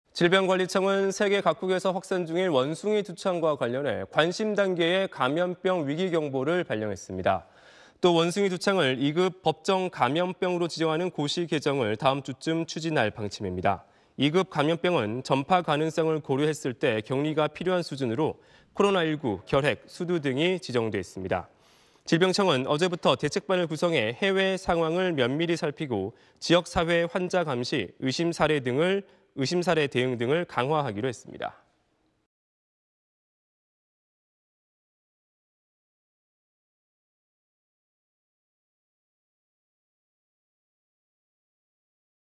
원숭이두창 '관심' 경보…2급 법정감염병 지정하기로 / SBS
질병관리청은 세계 각국에서 확산 중인 원숭이두창과 관련해 '관심 단계'의 감염병 위기 경보를 발령했습니다.
또 원숭이두창을 2급 법정감염병으로 지정하는 고시 개정을 다음주쯤 추진할 방침입니다.
2급 감염병은 전파 가능성을 고려했을때 격리가 필요한 수준으로, 코로나19, 결핵, 수두 등이 지정돼 있습니다.